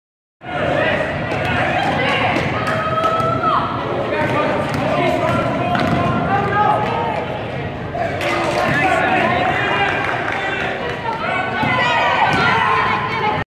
11-10-16 - Nick Beltran scores off the glass (Sterling 36, Fort Morgan 22)
Fort Morgan Colts at Sterling 8th grade basketball on November 10, 2016. Sterling defeated Fort Morgan, 36-22.